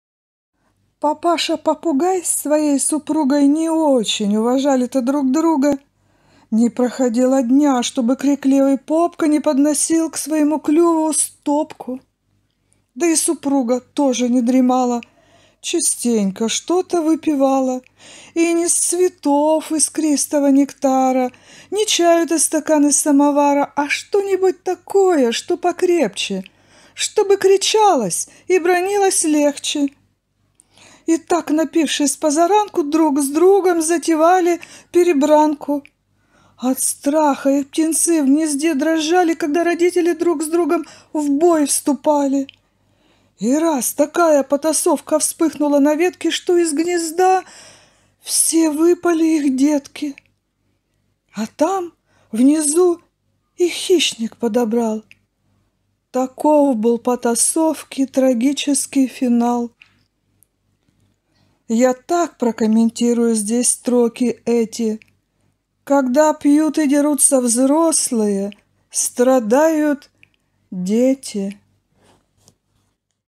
БАСНЯ "ПОПУГАЙ"
Басня Леонида Галуза "ПОПУГАЙ "
Выполнено Творческой Мастерской ТАЛЮТА.
с Канала Александра Бакуто
с канала Геннадия Титова.
Фото взяты из интернета в свободном доступе